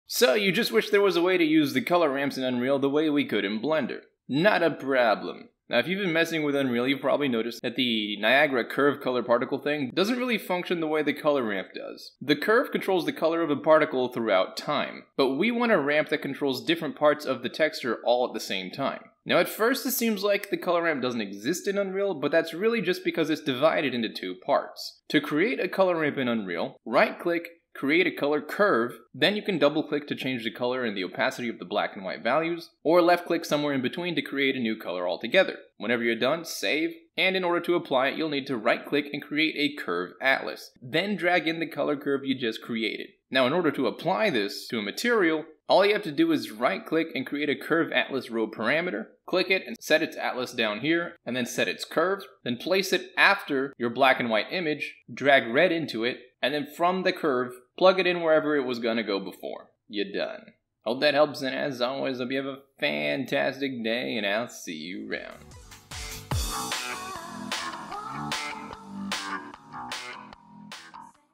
UE5: Color-Ramp Gradients (Curve Atlas & Color Curves)
If you've ever wondered how to add a "color-ramp" to Unrael5 similar to how we do in Blender, here's how it works -